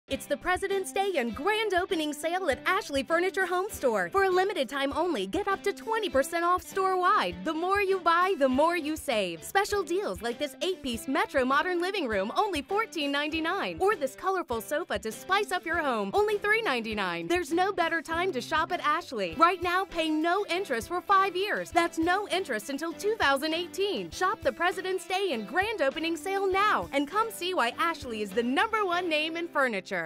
President's Day - Week 1
It's the President's Day Sale at Ashley Furniture HomeStore! PLUS - help us celebrate the GRAND OPENING of our brand new Raleigh, NC store with specials values on Mattresses & Furniture! OR get no interest until 2018! Hurry - These amazing deals won't last!